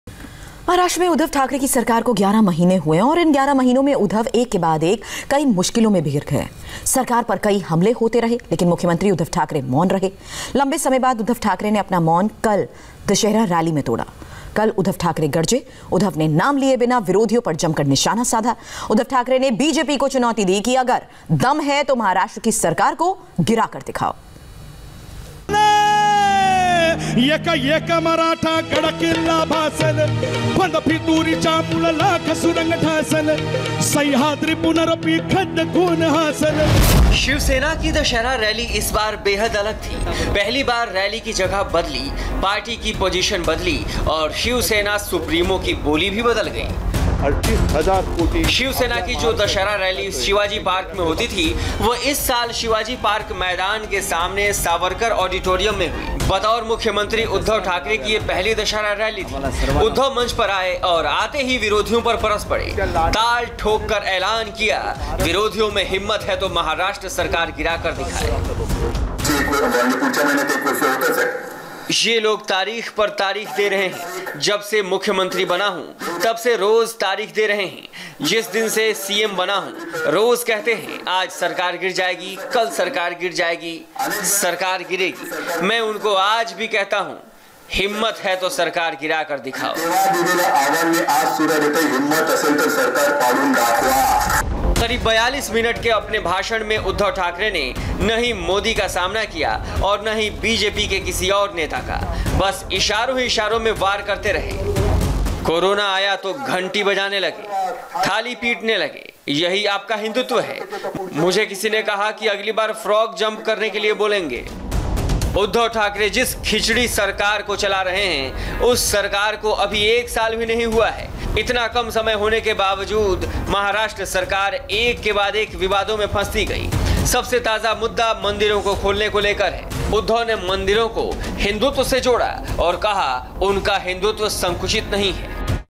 Uddhav Thackeray Slams BJP At Its Dussehra Rally, Alleges Attempts By Party To Topple Its Govt
Shiv Sena chief and Maharashtra CM Uddhav Thackeray on Sunday addressed the party's Dussehra rally for the first time after holding the post of state's chief minister at Savarkar hall in Dadar. While addressing his party members and workers he slammed the BJP.
Thackeray alleged that in last one year many attempts were made by the Centre to topple Maharashtra government but all were in vain.